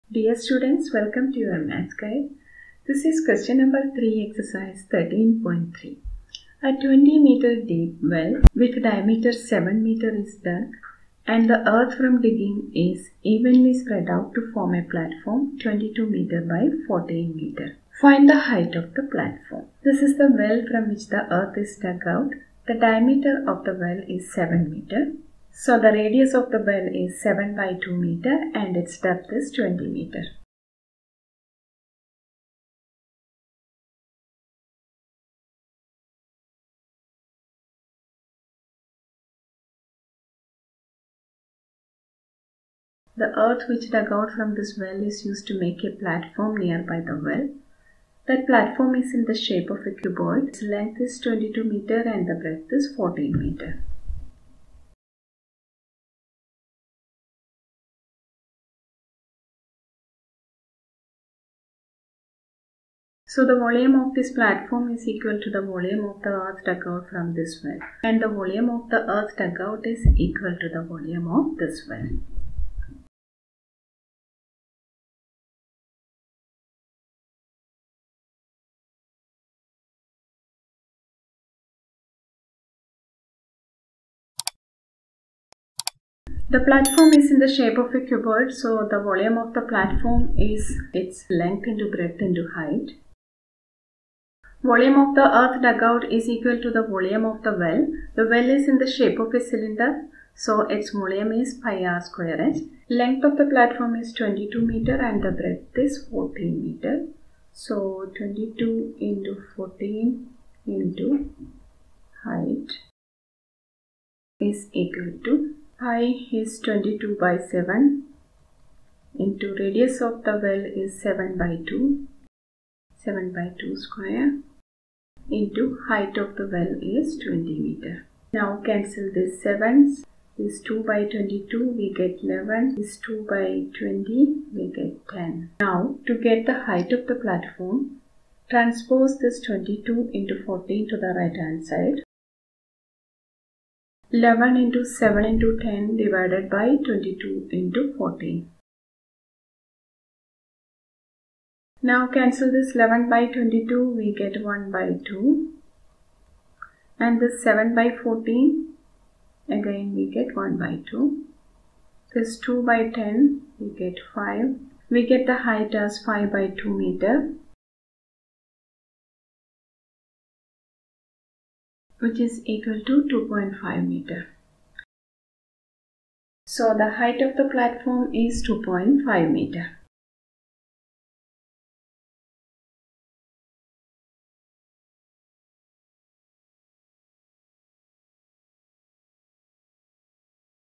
Class 10 | Ex 13.3 Question 3| A 20 m deep well with diameter 7 m is dug and the earth from........
A 20 m deep well with diameter 7 m is dug and the earth from digging is evenly spread out to form a platform 22 m by 14 m. Find the height of the platform.